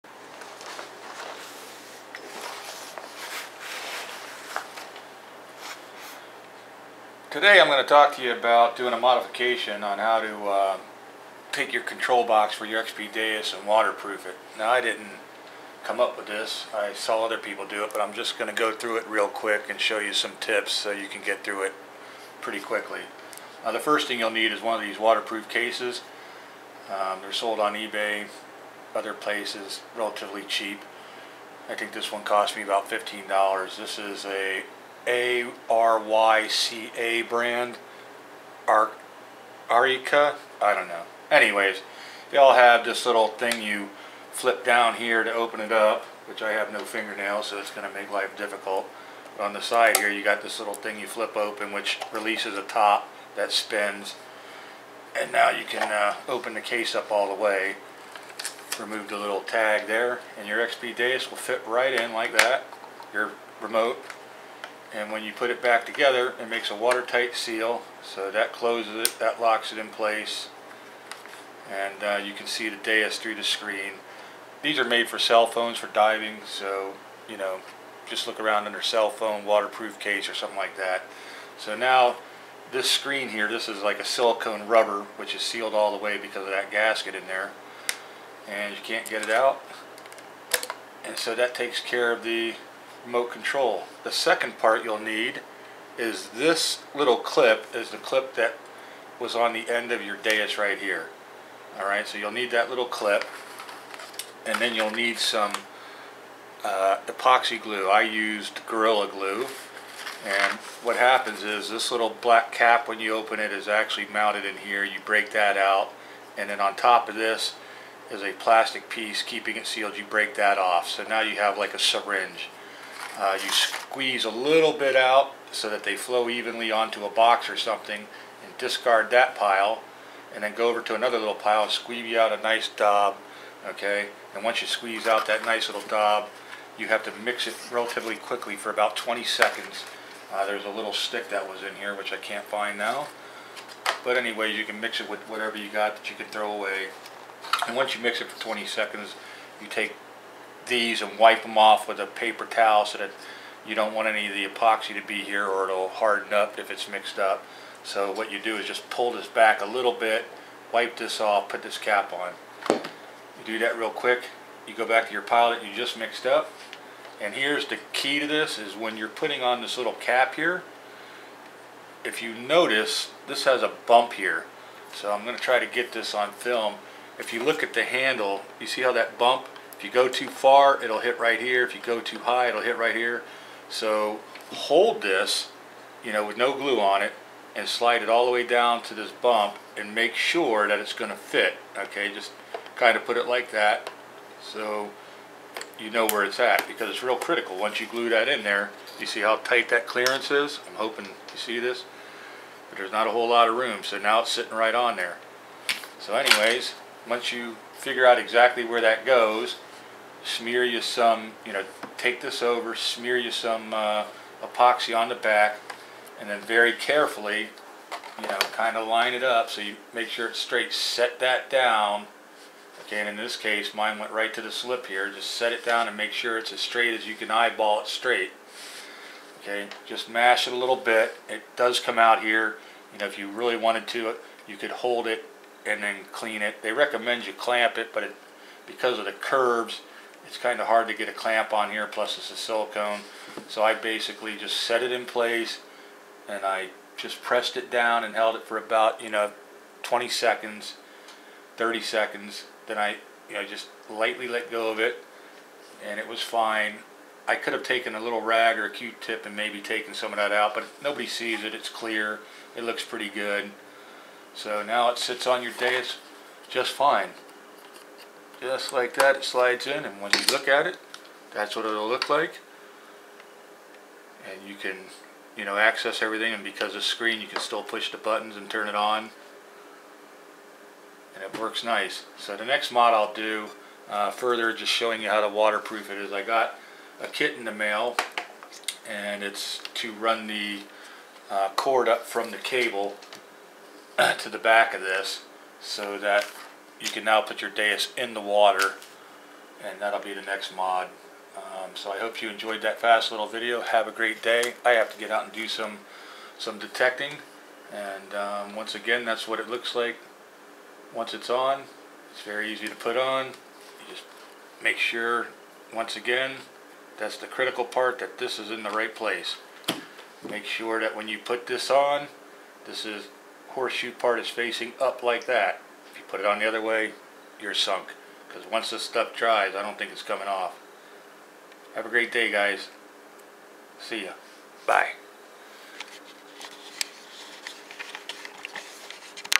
XP Deus Modification #1 Remote water proof Metal Detector Treasure Hunting
Xp Deus Metal Detector modification to make the metal detectors remote unit waterproof.  It is great when you are treasure hunting and metal detecting around water or when you want to keep the dirt and mund off your detector..